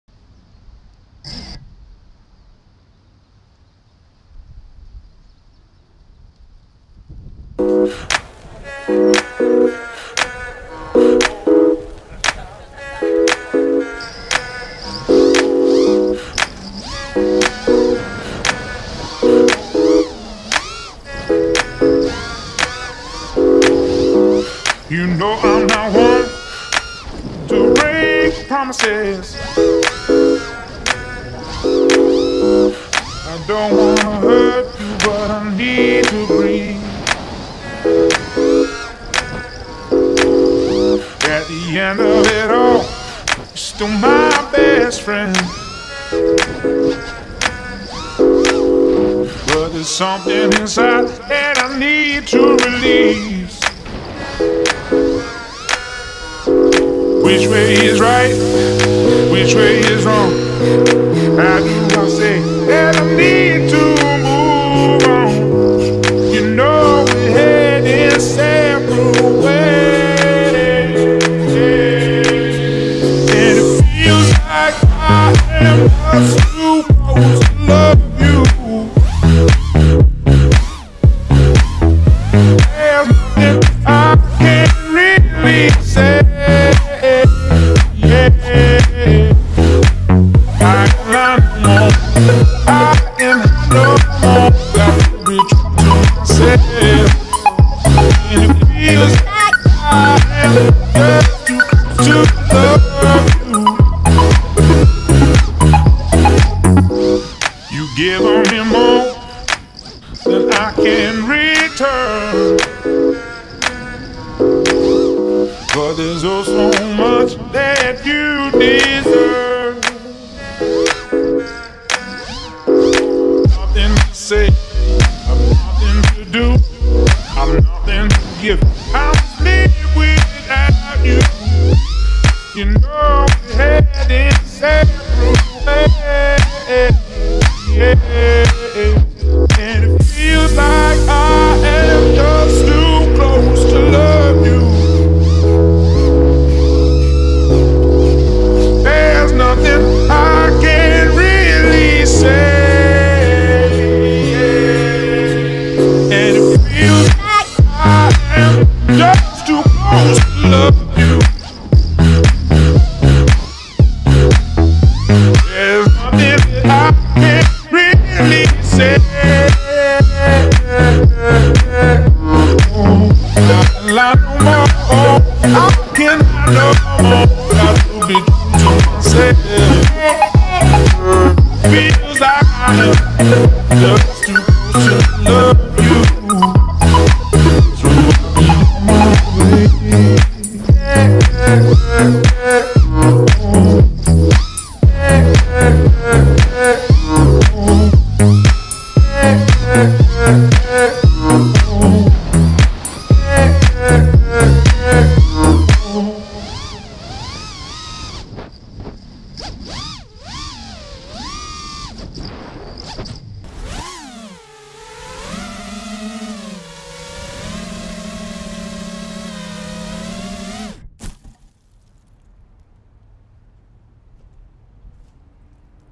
Stick with whatcha know.
Been trying some new moves and working on cleaning up the style of flying a bit but it just wasn't working for me so I'll just stick to my style I got right now. Hope you enjoy

Equipment

Radio: FrSky Taranis with TBS crossfire
RX: tbs micro rx
Frame: Dronewolf 5" Pup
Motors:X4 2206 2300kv
Props: Gemfan 5x4x3
Battery: Tattu 1300mah 4s
FC: Motolab Cyclone
ESC: KISS 24a
Flightcam: hs1177
Goggle: Fatshark HD2
HDcam: GoPro 4 Silver